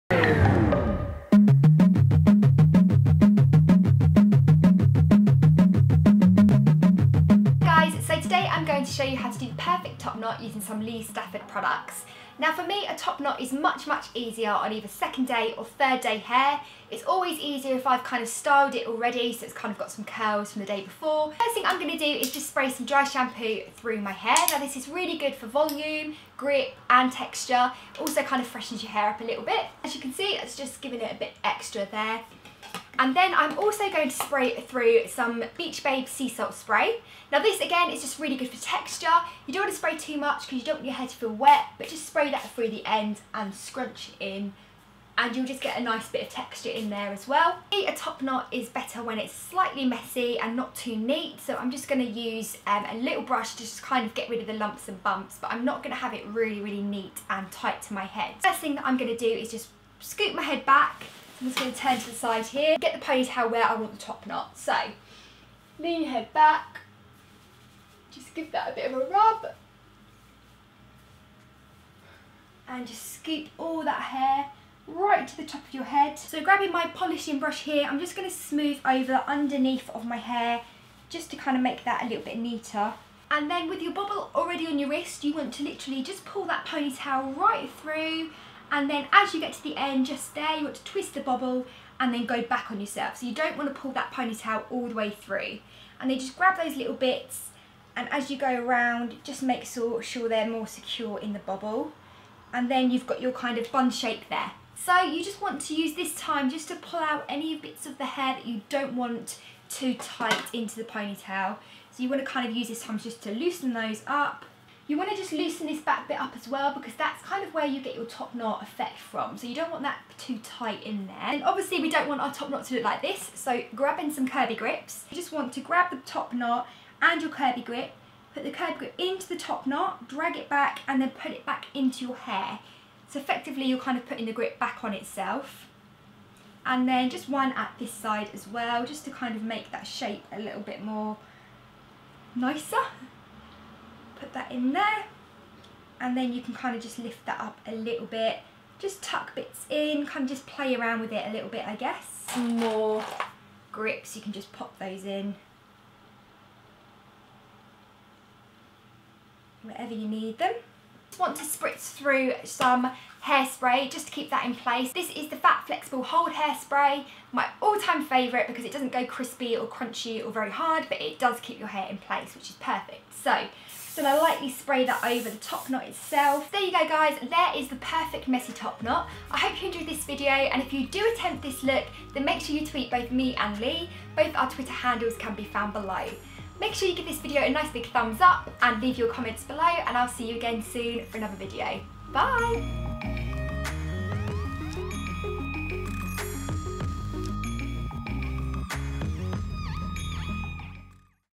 How To: The MeSSY ToP KnoT
Re-create the MeSSy ToP KnoT at home with this super simple HAiR tutorial.

Have Fun, Look Wicked, Smell BEAutiful xXx

SOCIAL MEDIA...